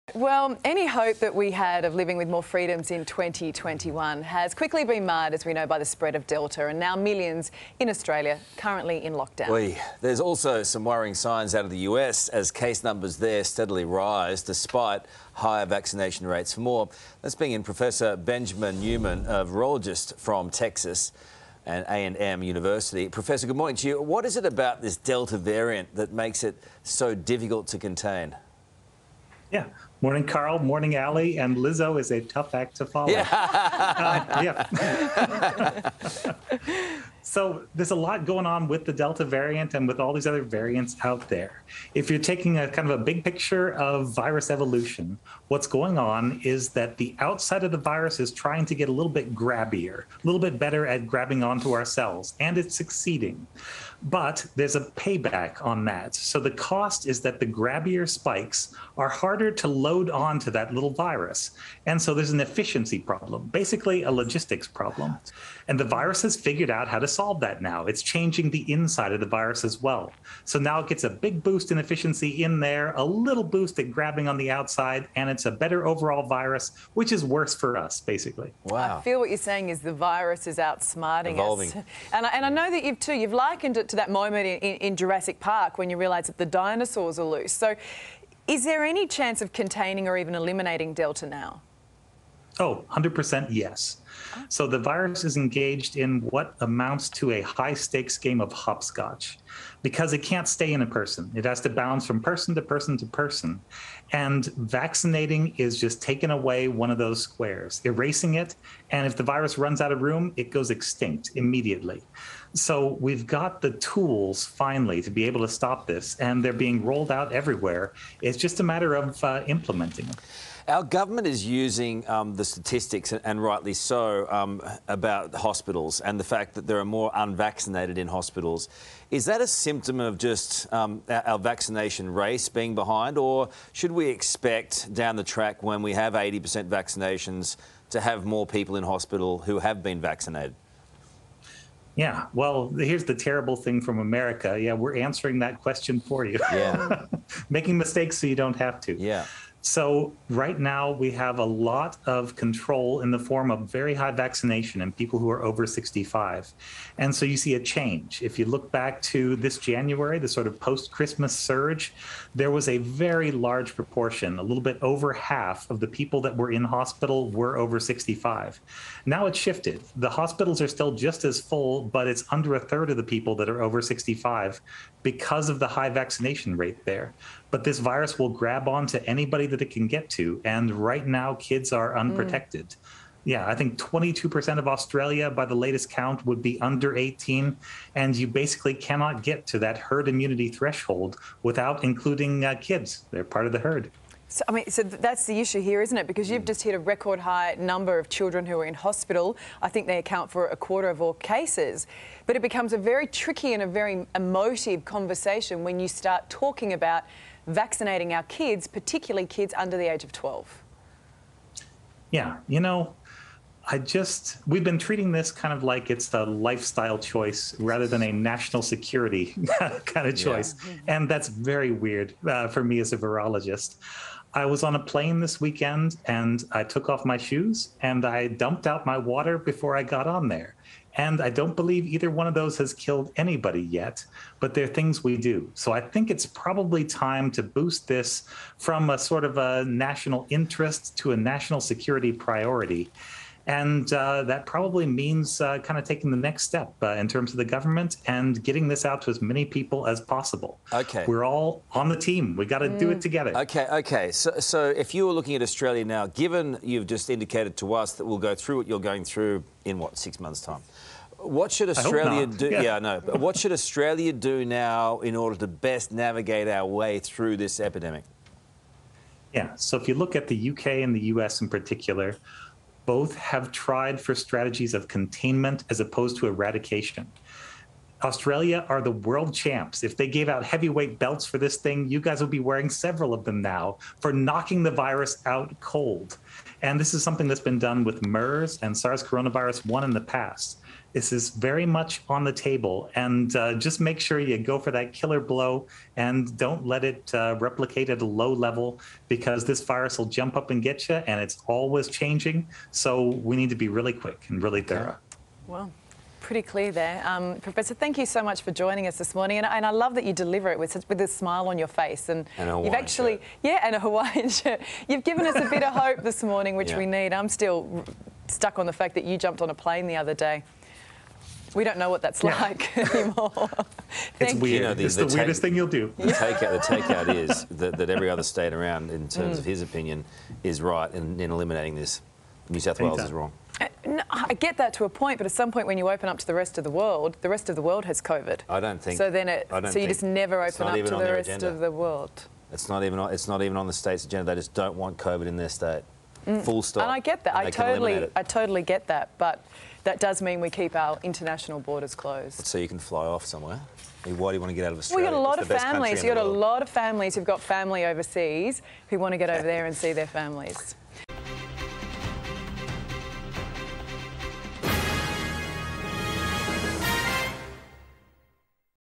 Expert calls for kids to be vaccinated against COVID-19 | Coronavirus | 9 News Australia
A virologist has said vaccinating children and maintaining a coronavirus elimination strategy will be key for Australia exiting the pandemic, as opposed to the US and UK’s containment approach.